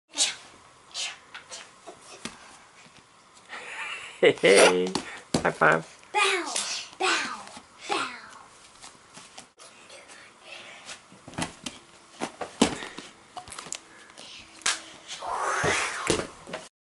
Feet to Feet Rolls
The kids have been practicing their front rolls and trying to stay tucked so they can go back up to their feet.  They call them 'feet to feet' since you start on your feet and end on your feet.

They made themselves a wedge/cheese to practice with out of the sofa cushions.  My daughter's sound effects when positioning herself are great too.